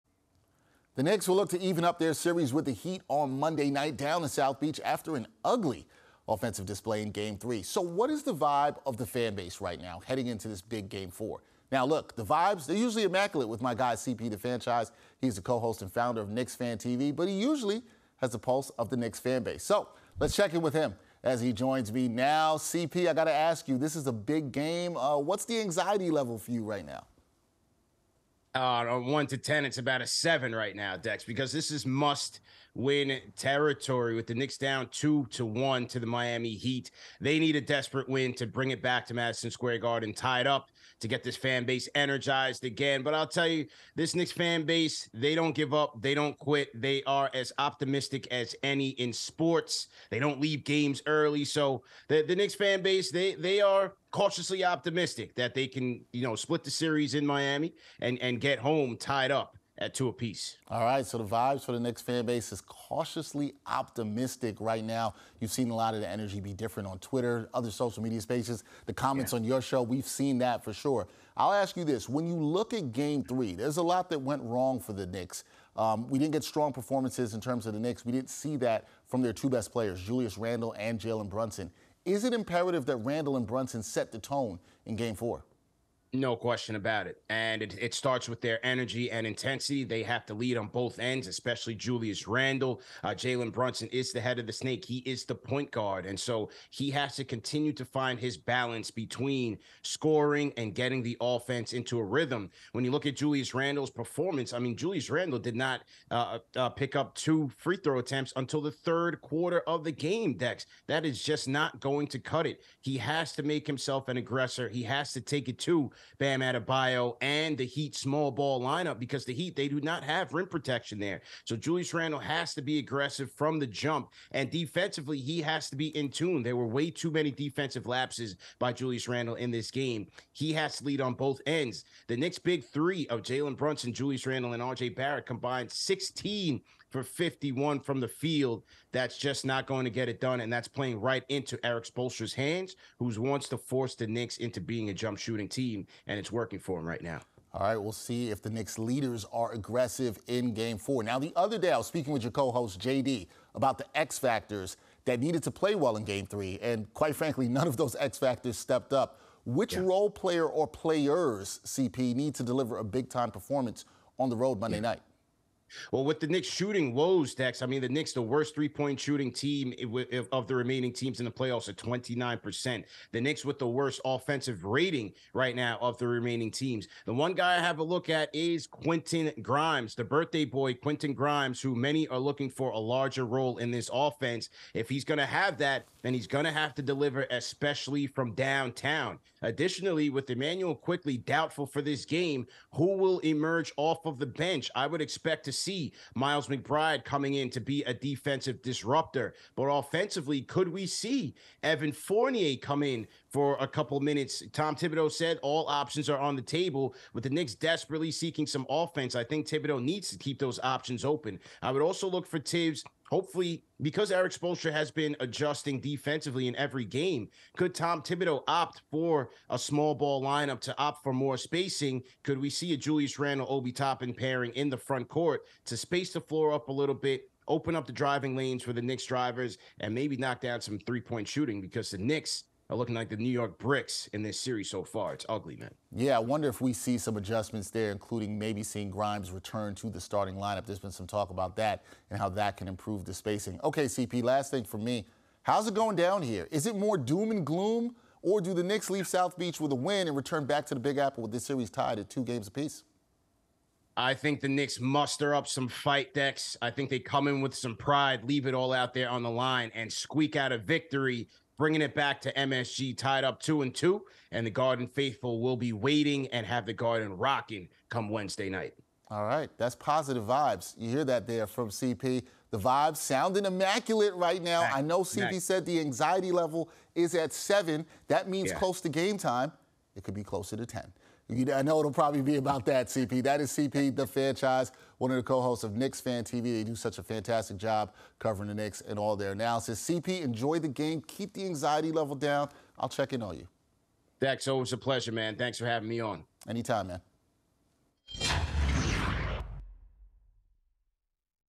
Which Knicks Player Needs A Big Performance In Game 4?! | SNYTV
The focus is on the New York Knicks as they aim to even up their series with the Miami Heat after an unimpressive offensive display in game three. The-host and founder of Knicks fan TV, CP The Fanchise joins SNY TV to discuss the vibe of the Knicks fan base, which is cautiously optimistic heading into game four. They also talk about the need for Julius Randle and Jalen Brunson to set the tone in the game and the importance of Quentin Grimes and other role players stepping up to deliver a big performance. The possibility of a small ball lineup is also explored as the Knicks seek some much-needed offense to get back into the series.

Catch CP hosting on SiriusXM NBA Radio Ch. 86